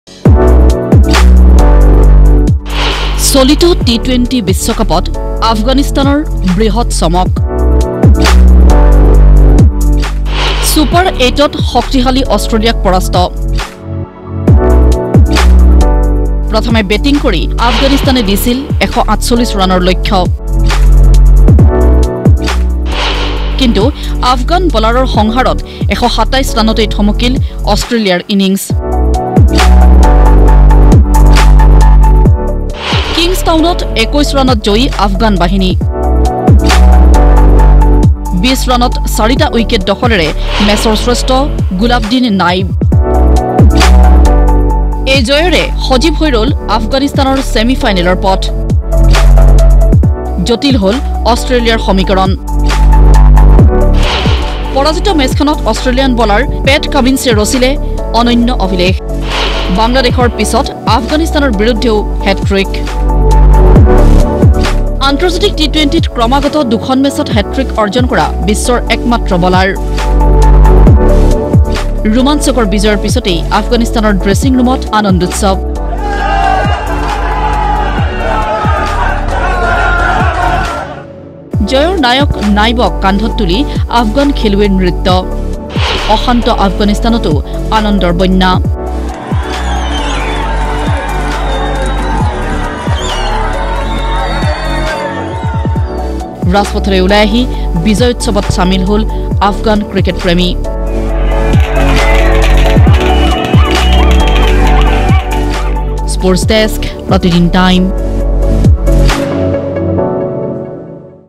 T-20 বিশ্বকাপত আফগানিস্তানৰ বৃহৎ চমক, ওলট-পালট সমীকৰণ.....
চলিত T-20 বিশ্বকাপত আফগানিস্তানৰ বৃহৎ চমক। ছুপাৰ এইটত শক্তিশালী অষ্ট্ৰেলিয়াক পৰাস্ত কৰি জীয়াই ৰাখিলে ছেমি ফাইনেলৰ সম্ভাৱনা। স্মৰণীয় জয়ৰ পিছতেই অশান্ত আফগানিস্তানত আনন্দৰ বন্যা।